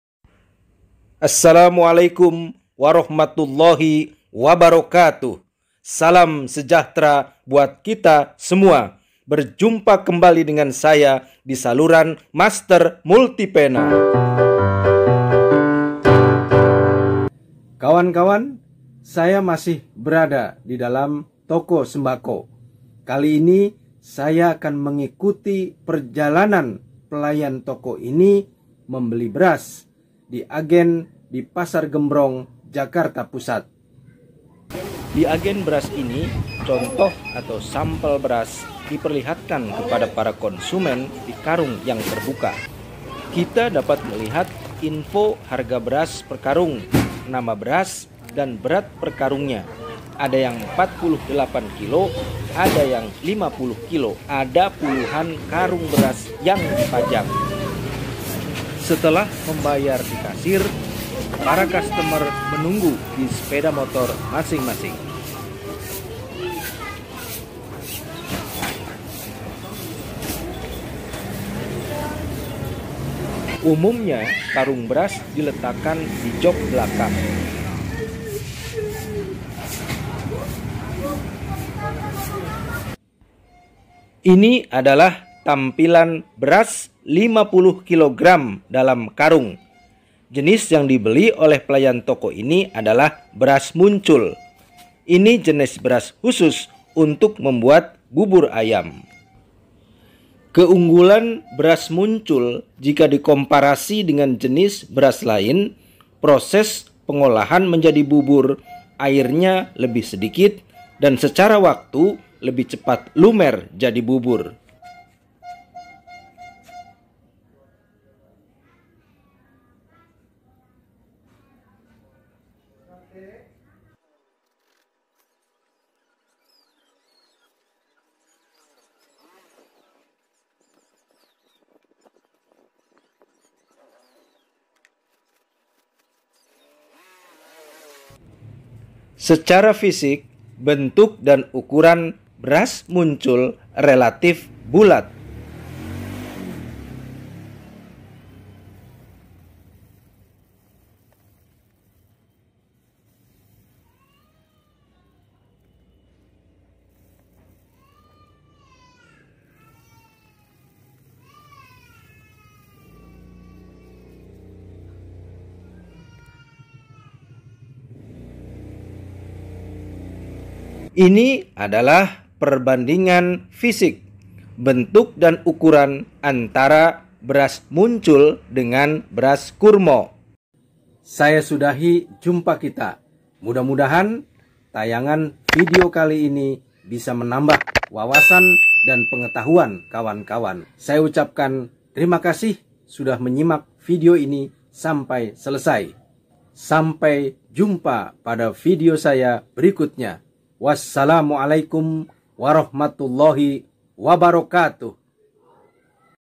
Mau Bikin Bubur Ayam ? Pilih Beras Muncul | Aktifitas Toko Sembako
Video ini menjelaskan pembelian beras muncul di Pasar Gembrong, Jakarta Pusat.